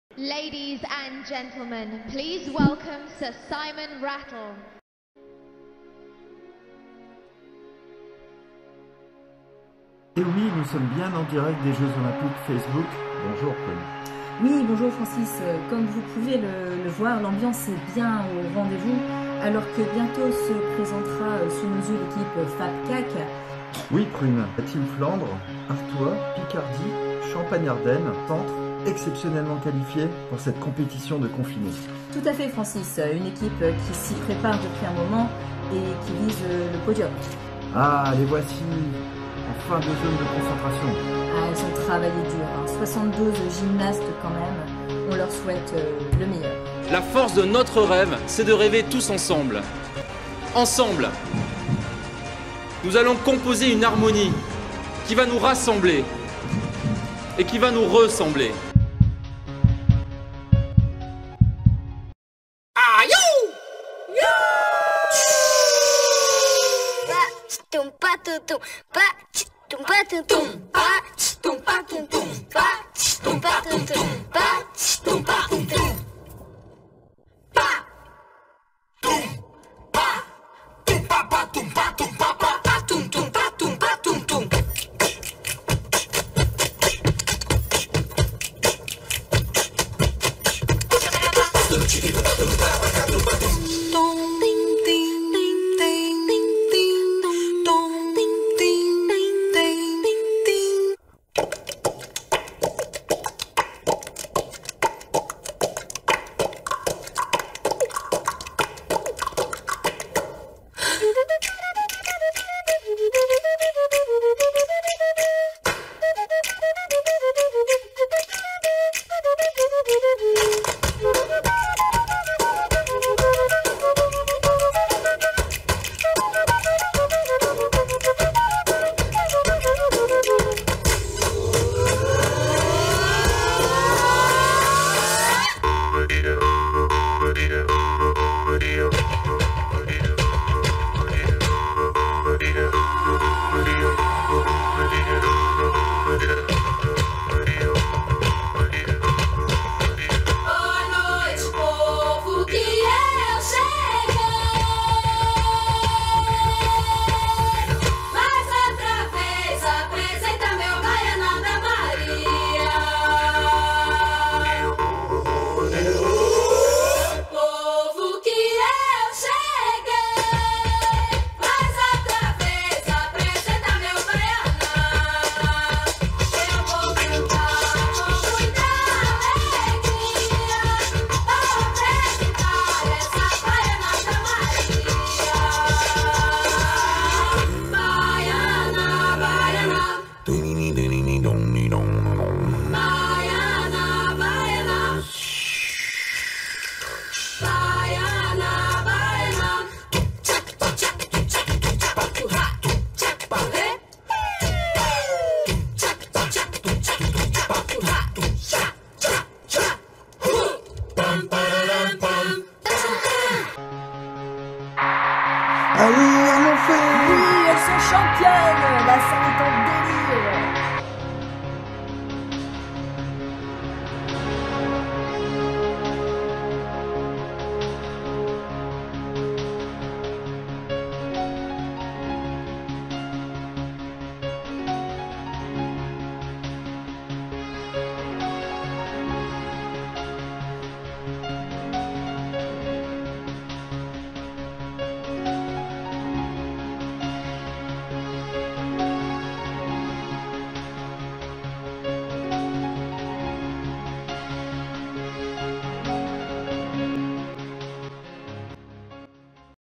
Défi 6 CSR GRS FAPCAC : Un ensemble tous ensemble
Retrouvez 9 clubs de la Région pour un seul et unique ensemble de GR !